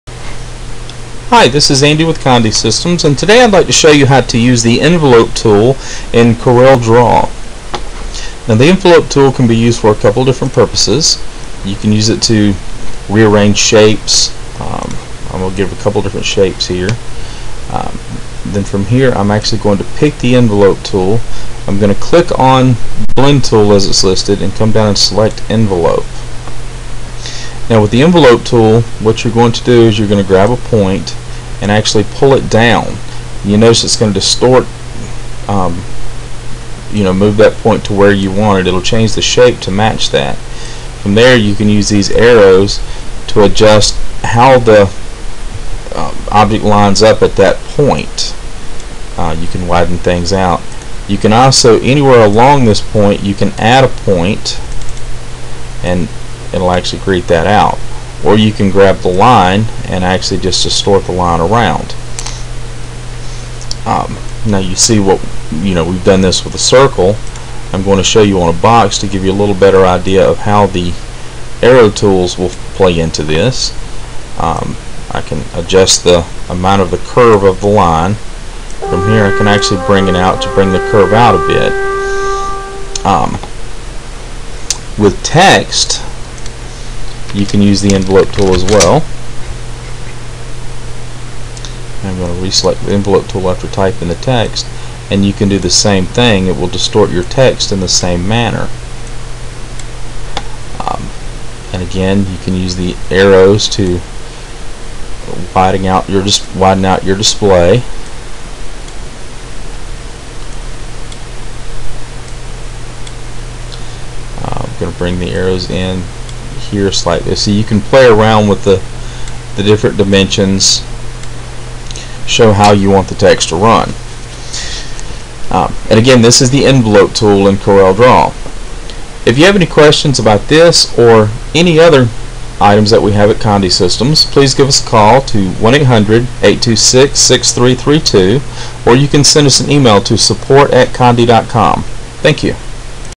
Using the Envelope Tool in CorelDRAW -
Conde Systems Tech Support Manager Andy Taylor shows how to use the Envelope Tool in CorelDRAW (most versions will work) when preparing your image for digital imprinting. This will work with almost any newer version of Corel. Graphic programs are essential to the imaged products industry and working with tools like Corel make the work easier and fun! Contact conde.com or call for information about working with personalized and imaged products created by dye sublimation and heat transfer applications.